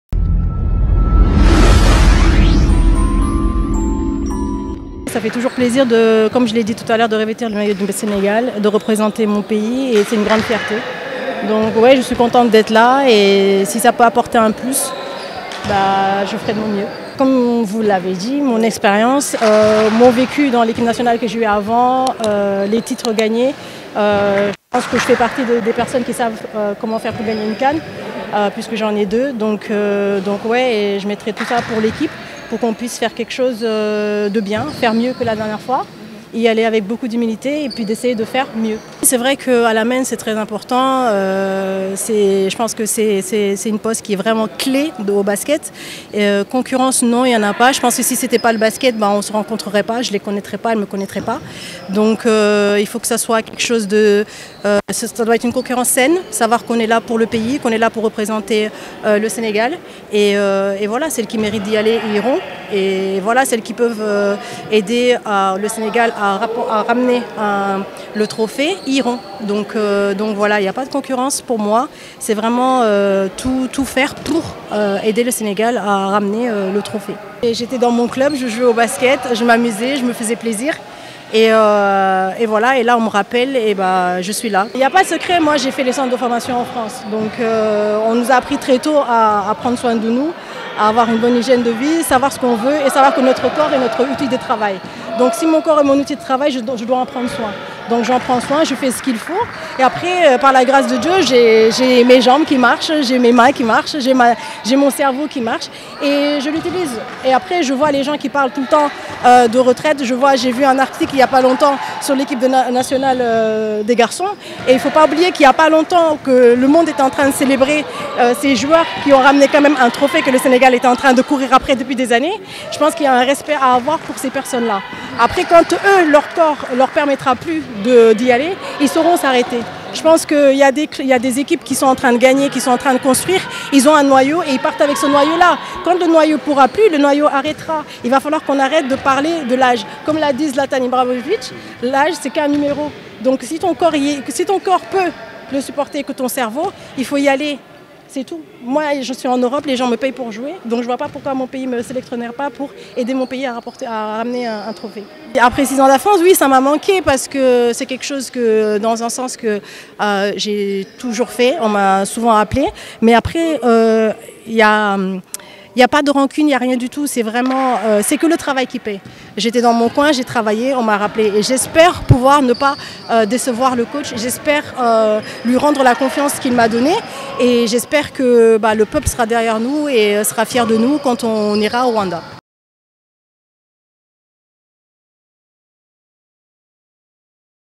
Afrobasket 2023 - Fatou Dieng : «Heureuse de revêtir le maillot national»
De retour dans la tanière, Fatou Dieng ne cache pas sa joie de revêtir le maillot du Sénégal. La meneuse championne d'Afrique s'exprimait aussi sur la préparation des lionnes pour l'Afrobasket 2023.